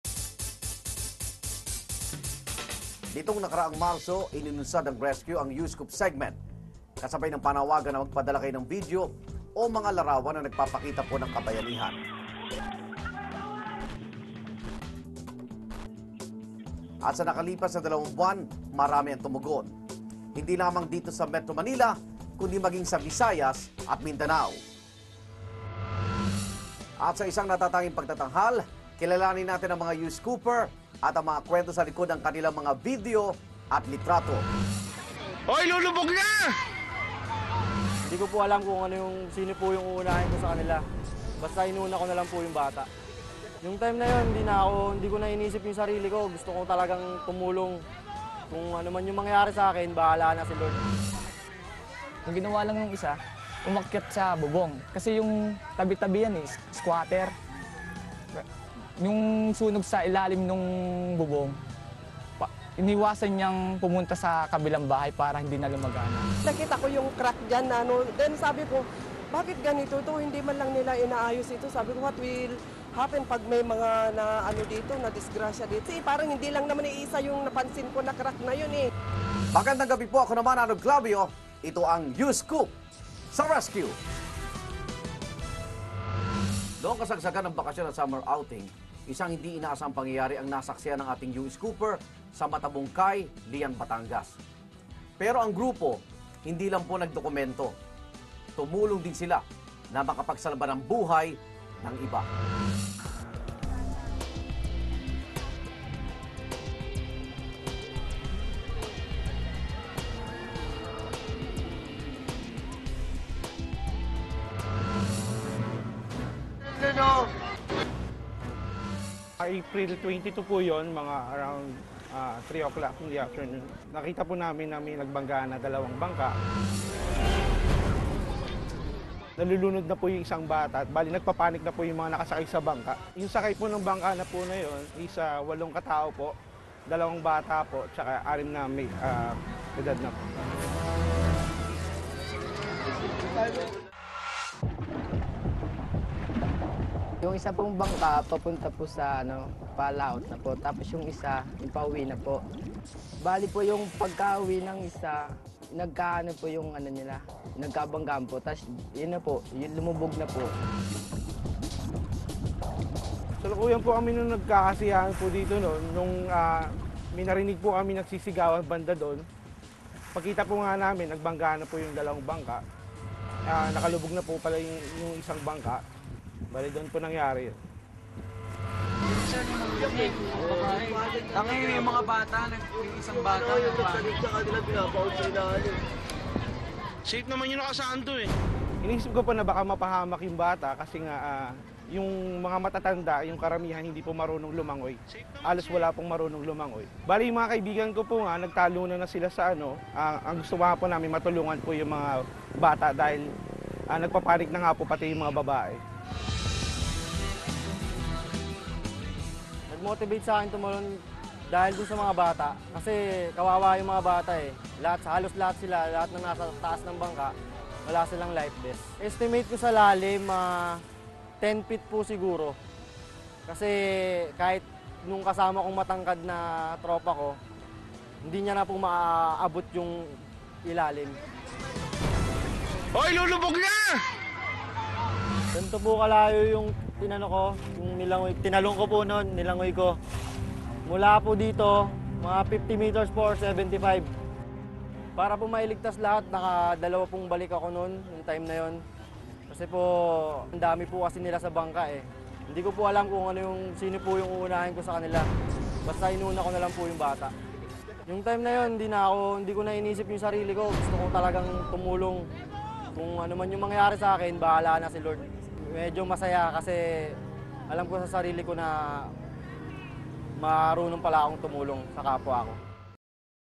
Rescue: Banggaan ng dalawang bangka sa isang bakasyunan, nakuhanan ng Youscooper
Sa kasagsagan ng bakasyon at summer outing, dalawang bangkang nagbanggan ang nakuhanan ng isang Youscooper habang nagbabakasyon sa Matabungkay beach sa Batangas.  Tampok sa video ang walang pag-aatubiling pagtulong ng ilang kalalakihan sa mga bata at matatandang lulan ng naaksidenteng bangka.  (Date aired:  May 10, 2012)